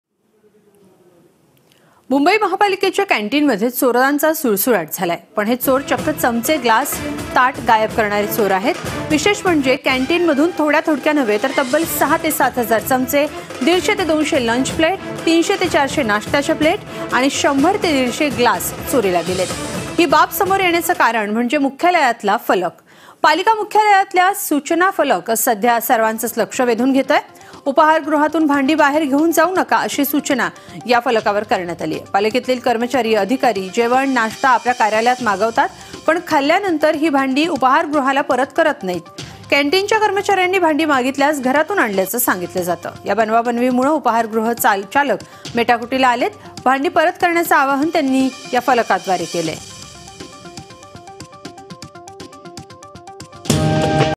Special Report | मुंबई महापालिकेच्या कॅन्टिनमध्ये चोरांचा सुळसुळाट, 7 हजार चमचे, 150 ग्लास गायब
Credit: Political | snehal jagtap /Producer